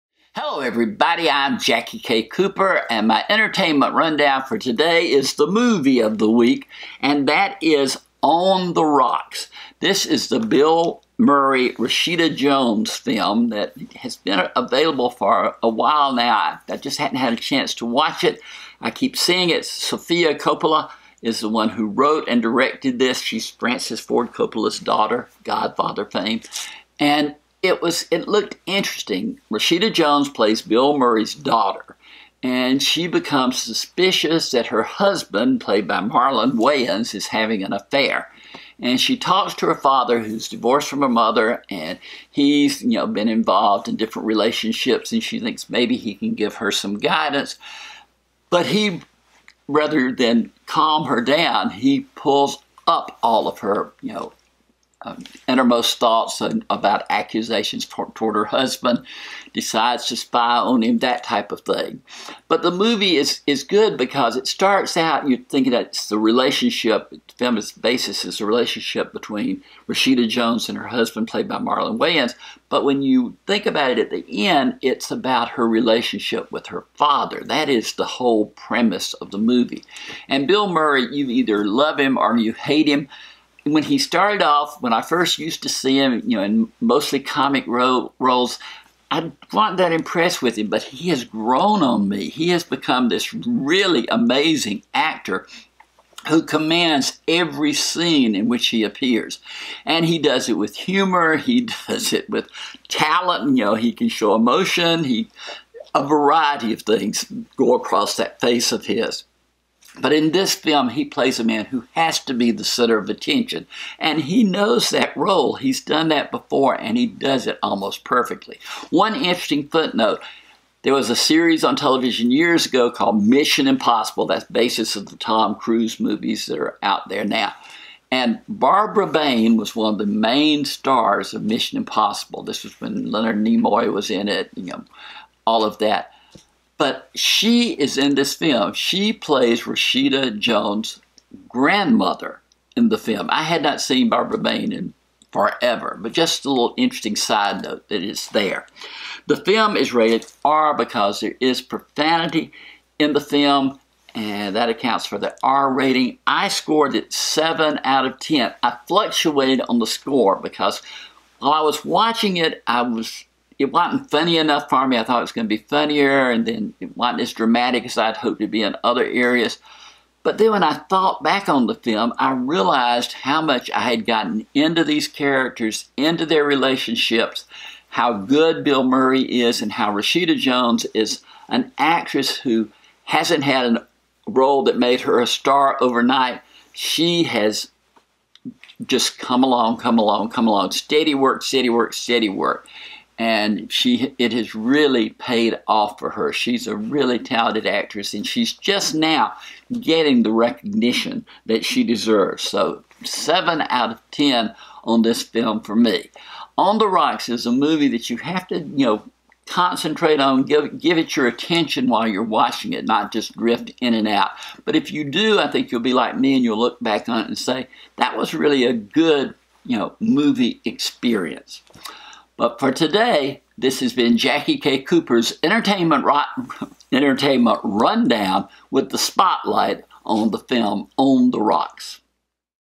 On the Rocks | Review
This is my review of the AppleTV Plus movie On the Rocks.